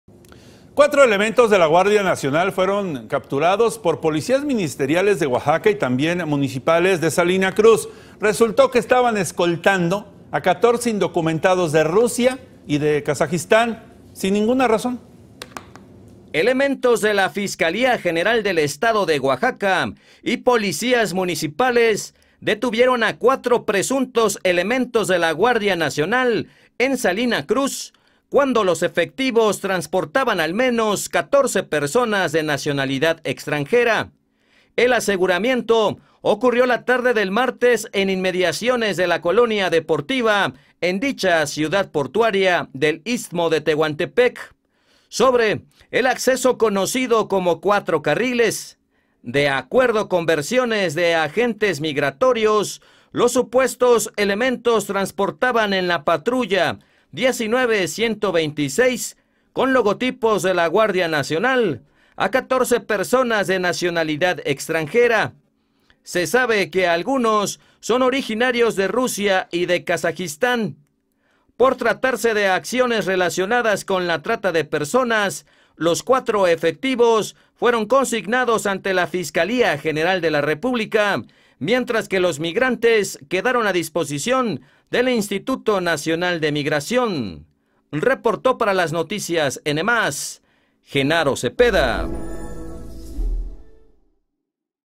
Guardias Nacionales Fueron Detenidos en Salina Cruz | Las Noticias Oaxaca -🔴 Noticias en vivo
Seguridad | Cuatro elementos de la Guardia Nacional fueron capturados por policías ministeriales y municipales en Salina Cruz, resultó que escoltaban a 14 indocumentados de Rusia y Kasajistan, sin dar razón alguna.

Con información de: Genaro Zepeda
La información mas importante de Oaxaca y sus 8 regiones en Las Noticias Oaxaca, de lunes a viernes de 8 a 9 pm / Síguenos en Facebook: NMás Oaxaca y  y en Twitter: @nmasoaxaca